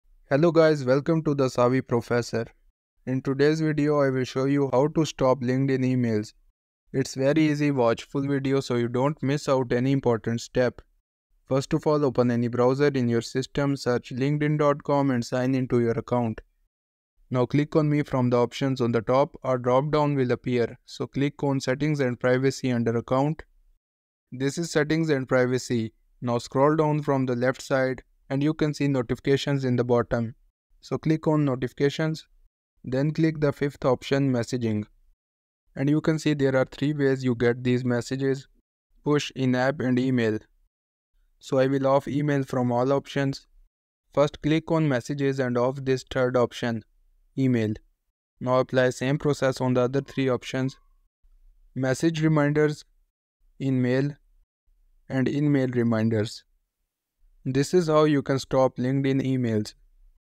How to Stop LinkedIn Emails (How to Unsubscribe from LinkedIn Emails)
How to Stop LinkedIn Emails (How to Unsubscribe from LinkedIn Emails). In this tutorial, you will learn how to stop LinkedIn Emails.